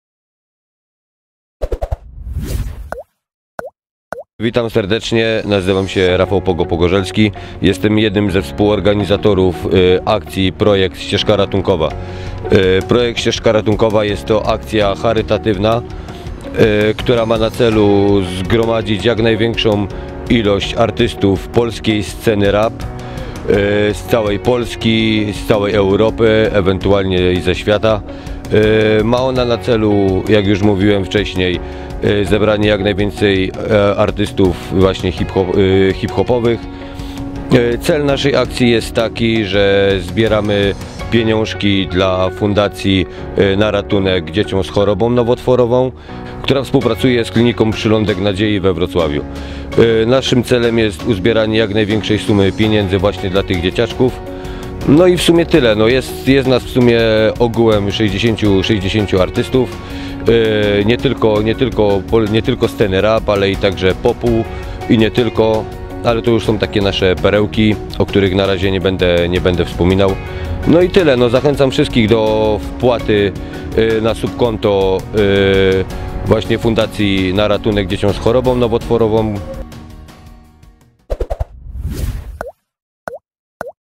ZAPOWIEDŹ PROJEKTU ŚCIEŻKA RATUNKOWA
Ścieżka Ratunkowa to wyjątkowy projekt, który połączył artystów sceny rap i nie tylko. Cel jest szczytny - pomoc dzieciom chorym na nowotwór.